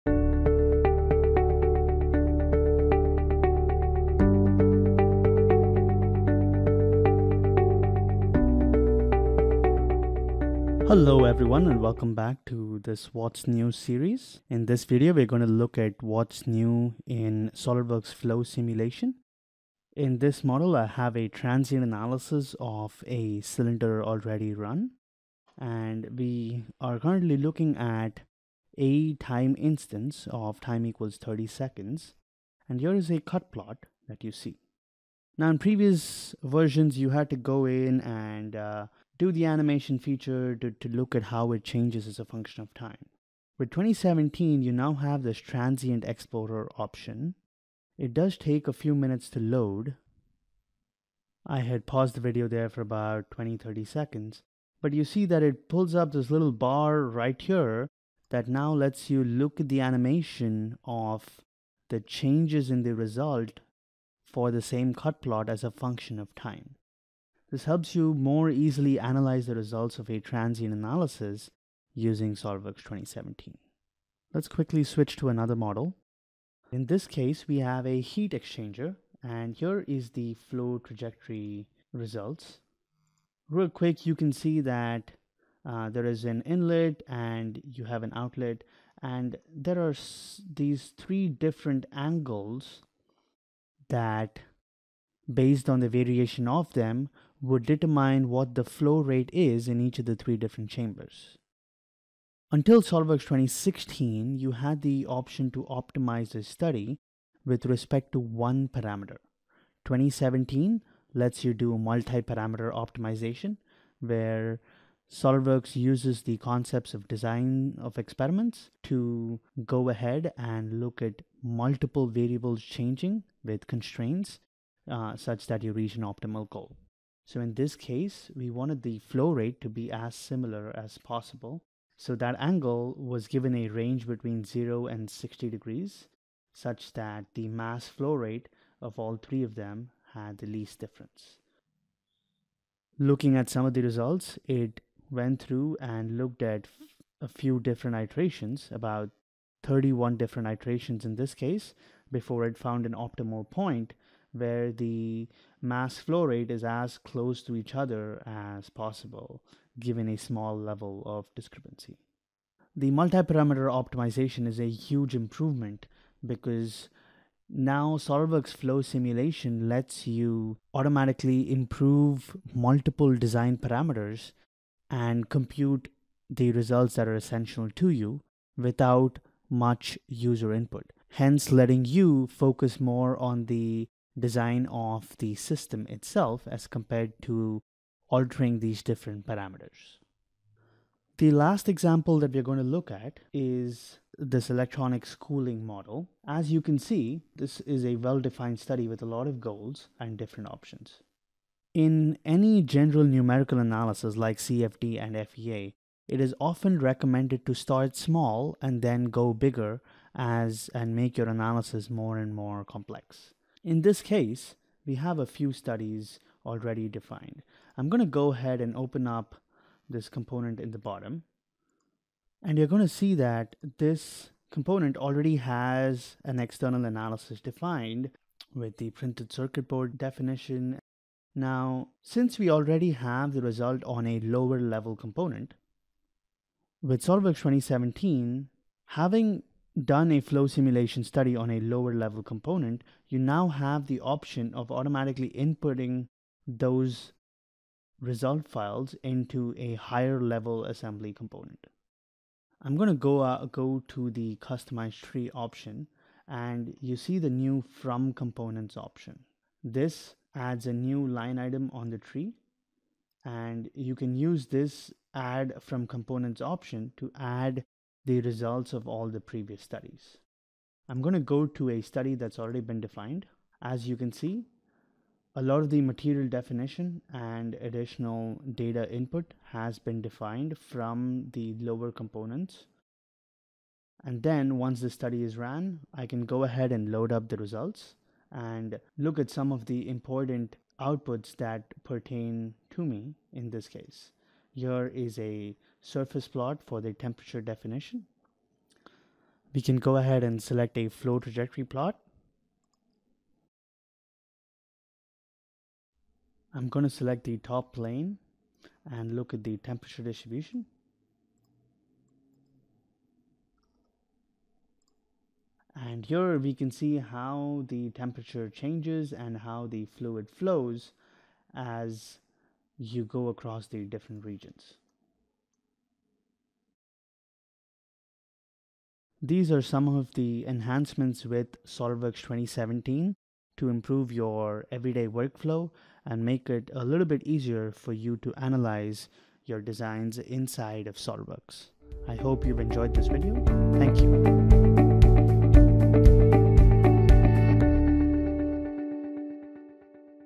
SOLIDWORKS 2017 - Flow Simulation
For SOLIDWORKS FLOW Simulation 2017, there are two significant enhancements that offer tremendous power to users.

The first enhancement is using FLOW conditions from a lower level component (part or assembly) in a higher level assembly.  In this case, imagine first analyzing the PCB board itself or even a heat sink you are attempting to optimize!  When you are ready to move to the higher level, in this case the full electronics enclosure, you no longer need to recreate the conditions of the PCB – you can simply import them from the model.

The second major enhancement to SOLIDWORKS Flow Simulation 2017 is the ability to setup Multi-Parameter Optimization studies.  Previous versions allowed only a single parameter.  With 2017, multiple parameters, both SOLIDWORKS CAD geometry and FLOW Simulation variables, can be used as input.